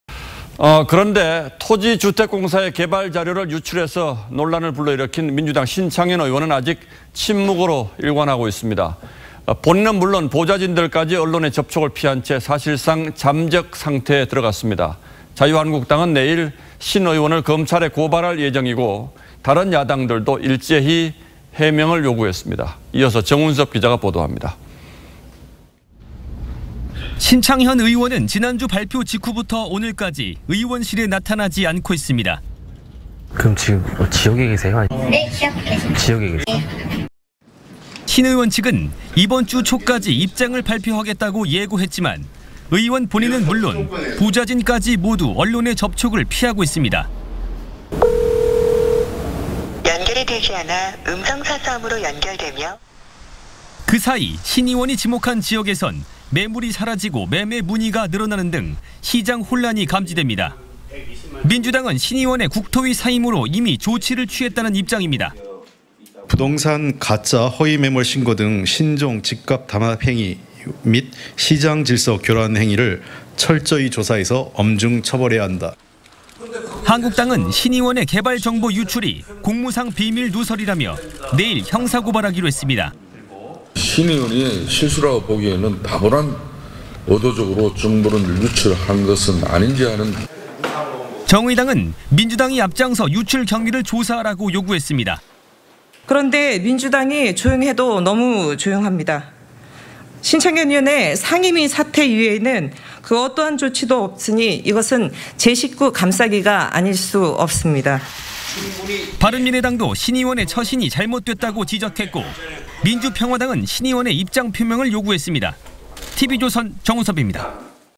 입장 발표 예고했던 신창현 '침묵'…야당 "11일 검찰 고발"
토지 주택공사의 개발 자료를 유출해서 논란을 불러 일으킨 민주당 신창현 의원은 아직 침묵으로 일관하고 있습니다. 본인은 물론 보좌진들까지 언론의 접촉을 피한채 사실상 잠적 상태에 들어 갔습니다. 자유한국당은 내일 신의원을 검찰에 고발할 예정이고 다른 야당들도 일제히 해명을 요구했습니다.